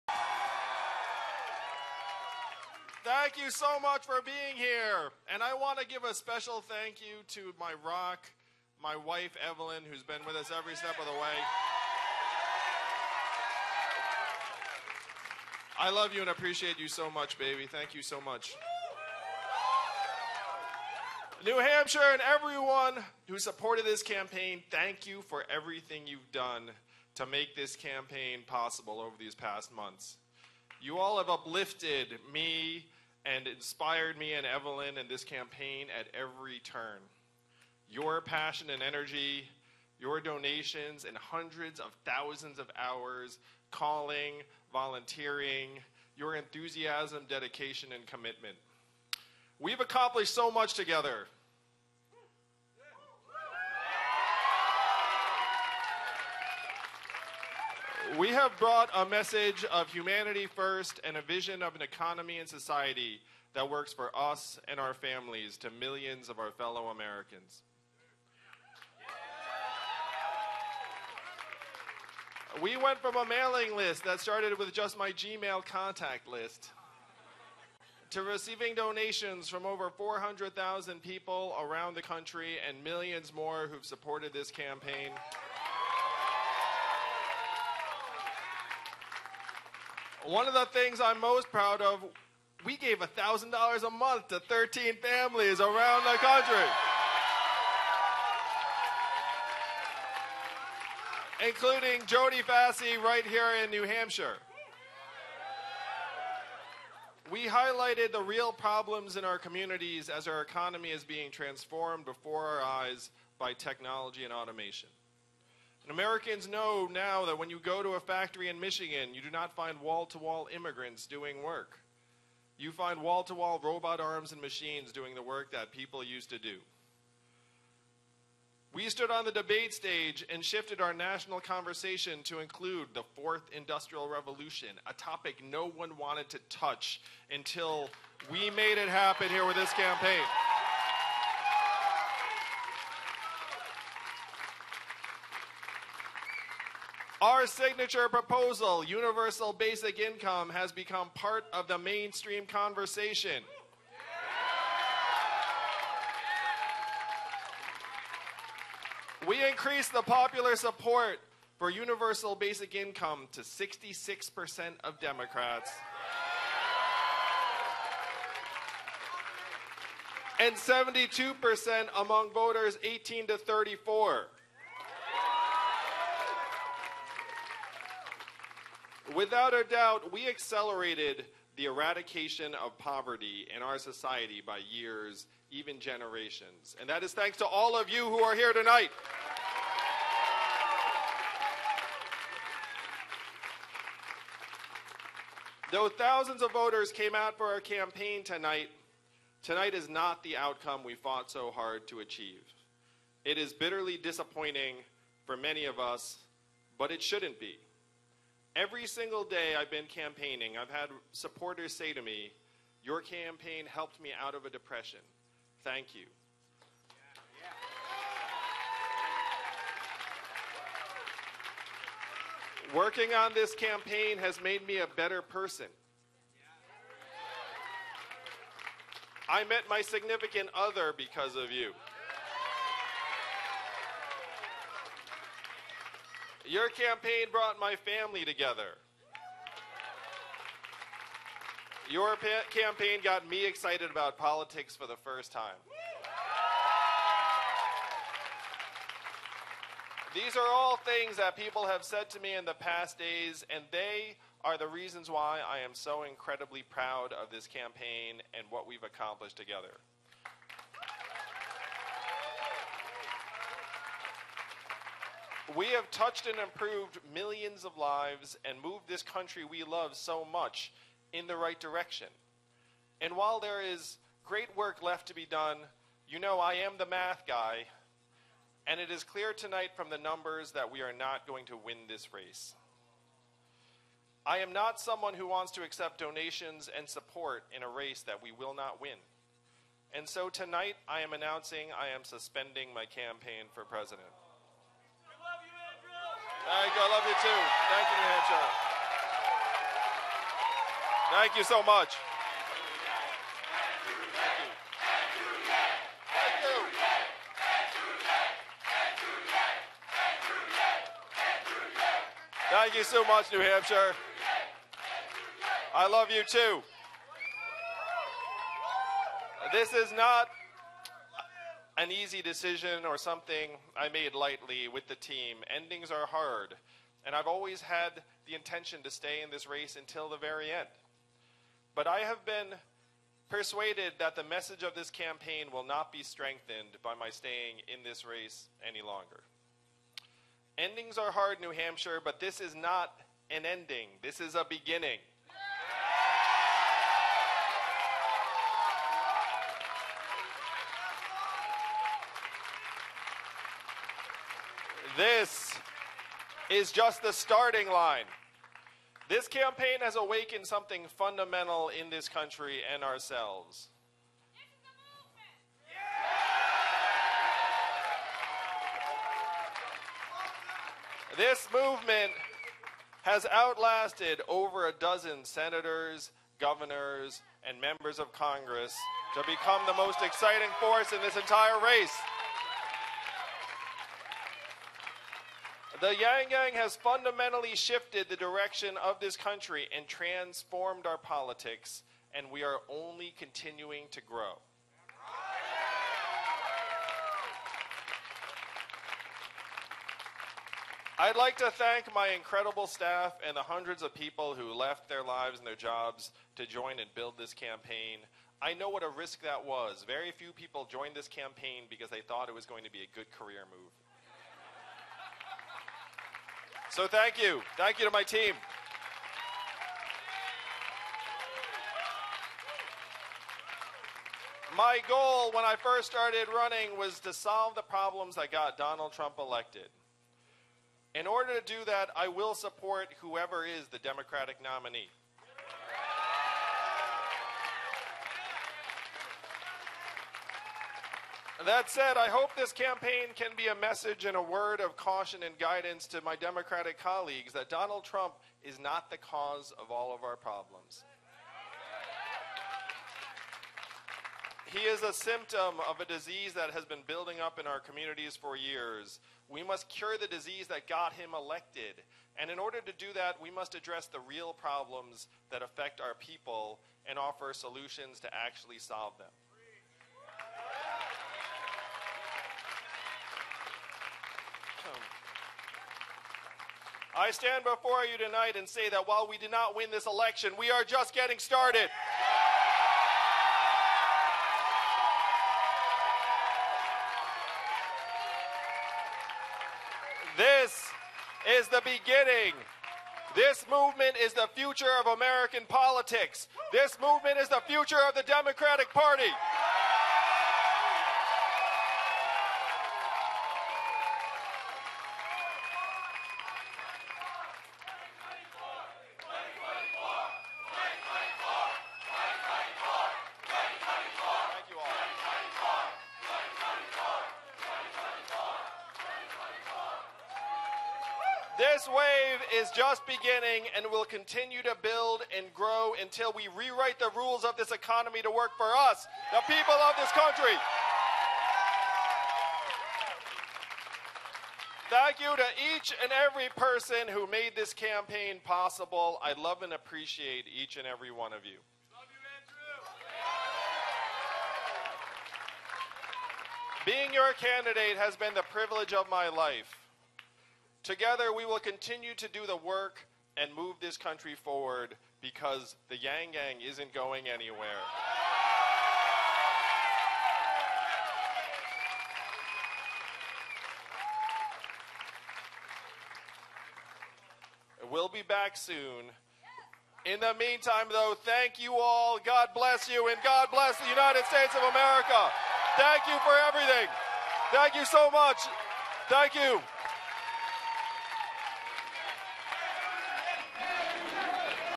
ANDREW YANG SUSPENDS 2020 campaign amidst New Hampshire results, supporters chant '2024!'
"The YangGang isn't going anywhere."

As the New Hampshire primary results came in, Andrew Yang suspended his campaign, but supporters made clear they hope he has a continued presence in politics.